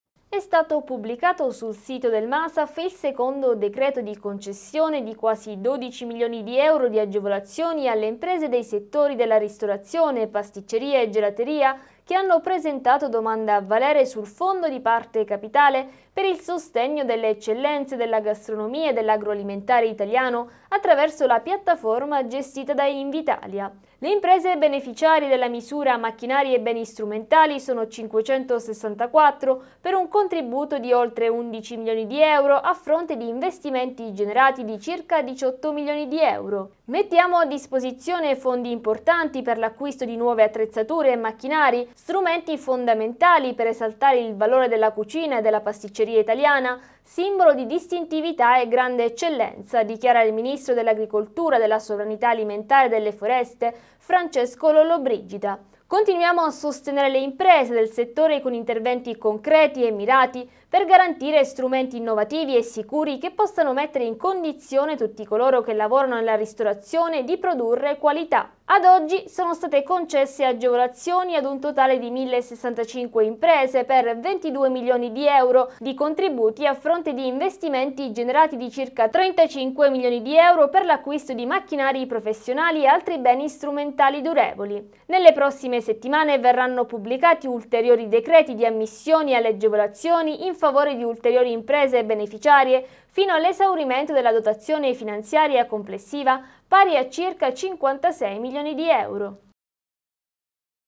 Fondo per le eccellenze gastronomiche: dal Masaf altri 12 milioni
Fondo per le eccellenze gastronomiche: dal Ministero dell'agricoltura, della sovranità alimentare e delle foreste (Masaf) altri 12 milioni